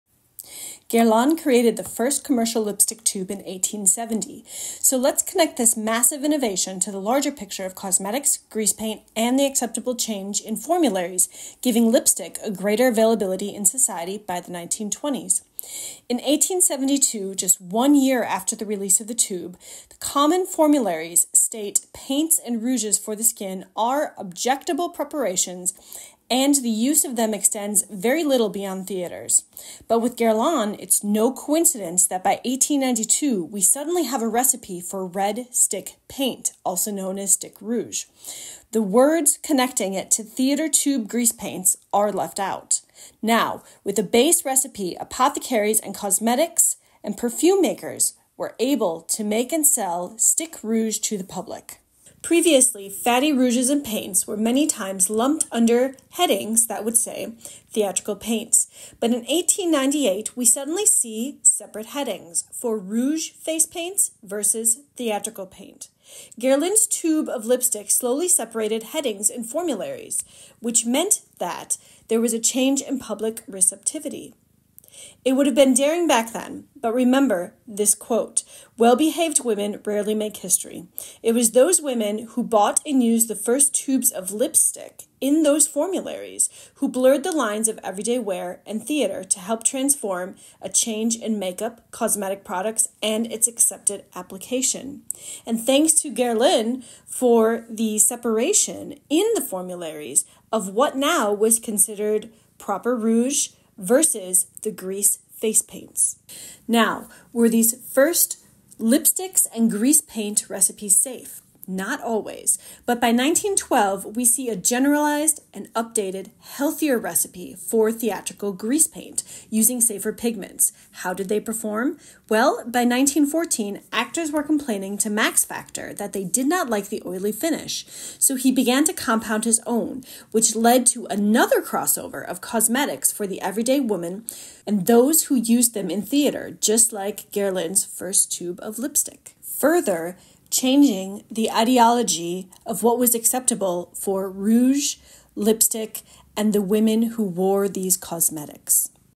Guerlain, Max Factor, Formularies & Theater Grease Paints- A Forward Step To The Future Of Lipsticks
This video connects the massive innovation of the first tube of lipstick to the larger picture of cosmetics, grease paints, and the change in formularies - changing the world of makeup and ushering in the 1920s.